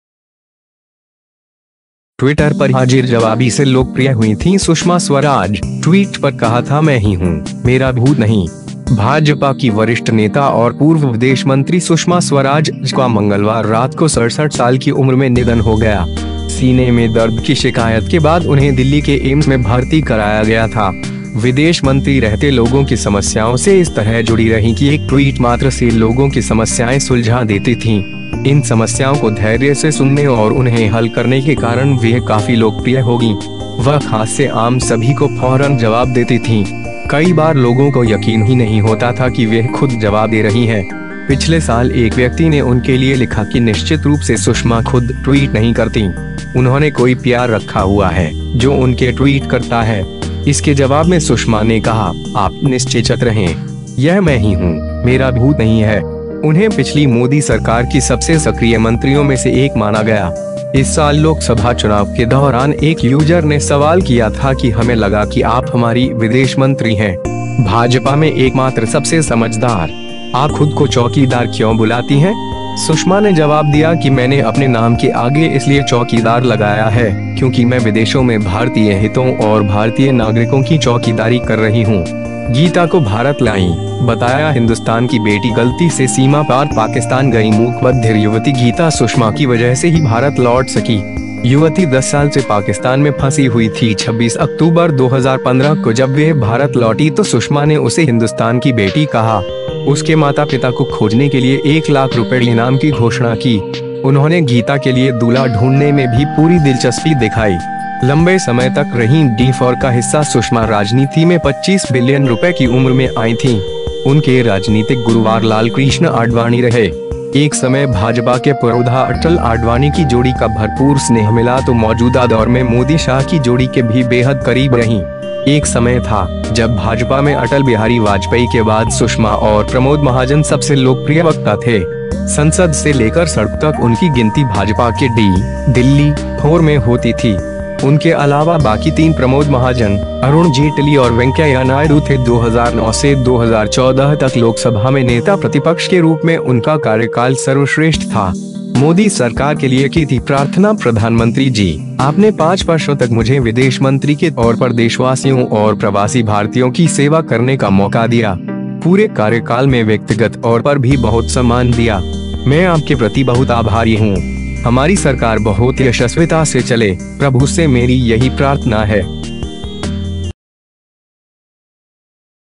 ट्विटर पर हाजिरजवाबी से लोकप्रिय हुईं थीं सुषमा स्वराज, ट्वीट पर कहा था 'मैं ही हूं, मेरा भूत नहीं'
भाजपा की वरिष्ठ नेता और पूर्व विदेश मंत्री सुषमा स्वराज का मंगलवार रात को 67 साल की उम्र में निधन हो गया। सीने में दर्द की शिकायत के बाद उन्हें दिल्ली के एम्स में भर्ती कराया गया था। विदेश मंत्री रहते लोगों की समस्याओं से इस तरह जुड़ी रहीं किएक ट्वीट मात्र से लोगों की समस्याएं सुलझा देती थीं। इन समस्याओं को धैर्य से सुनने और उन्हें हल करने के कारण वह काफी लोकप्रिय हो गईं। वह खास से आम सभी को फौरन जवाब देती थीं। कई बार लोगों को यकीन ही नहीं होता था कि वह खुद जवाब दे रही हैं।

पिछले साल एक व्यक्ति ने उनके लिए लिखा कि निश्चित रूप से सुषमा खुद ट्वीट नहीं करतीं। उन्होंने कोई पीआर रखा हुआ है, जो उनके ट्वीट करता है। इसके जवाब में सुषमा ने कहा, आप निश्चिंचत रहें। यह मैं ही हूं, मेरा भूत नहीं है। उन्हें पिछली मोदी सरकार की सबसे सक्रिय मंत्रियों में से एक माना गया। इस साल लोकसभा चुनाव के दौरान एक यूजर ने सवाल किया था कि हमें लगा कि आप हमारी विदेश मंत्री हैं। भाजपा में एकमात्र सबसे समझदार। आप खुद को चौकीदार क्यों बुलाती हैं? सुषमा ने जवाब दिया  कि मैंने अपने नाम के आगे इसलिए चौकीदार लगाया है, क्योंकि मैं विदेशों में भारतीय हितों और भारतीय नागरिकों की चौकीदारी कर रही हूं।

गीता को भारत लाईं, बताया हिंदुस्तान की बेटी
गलती से सीमा पार पाकिस्तान गई मूकबधिर युवती गीता सुषमा की वजह से ही भारत लौट सकी। युवती 10 साल से पाकिस्तान में फंसी हुई थी। 26 अक्तूबर 2015 को जब वह भारत लौटी तो सुषमा ने उसे हिंदुस्तान की बेटी कहा। उसके माता-पिता को खोजने के लिए एक लाख रुपये इनाम की घोषणा की। उन्होंने गीता के लिए दूल्हा ढूंढने में भी पूरी दिलचस्पी दिखाई।

लंबे समय तक रहीं डी-फोर का हिस्सा
सुषमा राजनीति में 25 बरस की उम्र में आईं थीं। उनके राजनीतिक गुरु लालकृष्ण आडवाणी रहे। एक समय भाजपा के पुरोधा अटल-आडवाणी की जोड़ी का भरपूर स्नेह मिला तो मौजूदा दौर में मोदी-शाह की जोड़ी के भी बेहद करीब रहीं। एक समय था, जब भाजपा में अटल बिहारी वाजपेयी के बाद सुषमा और प्रमोद महाजन सबसे लोकप्रिय वक्ता थे। संसद से लेकर सड़क तक उनकी गिनती भाजपा के डी (दिल्ली)-फोर में होती थी। उनके अलावा बाकी तीन प्रमोद महाजन, अरुण जेटली और वेंकैया नायडू थे। 2009 से 2014 तक लोकसभा में नेता प्रतिपक्ष के रूप में उनका कार्यकाल सर्वश्रेष्ठ था।

मोदी सरकार के लिए की थी प्रार्थना
प्रधानमंत्री जी,  आपने 5 वर्षों तक मुझे विदेश मंत्री के तौर पर देशवासियों और प्रवासी भारतीयों की सेवा करने का मौका दिया। पूरे कार्यकाल में व्यक्तिगत तौर पर भी बहुत सम्मान दिया। मैं आपके प्रति बहुत आभारी हूं। हमारी सरकार बहुत यशस्विता से चले, प्रभु से मेरी यही प्रार्थना है।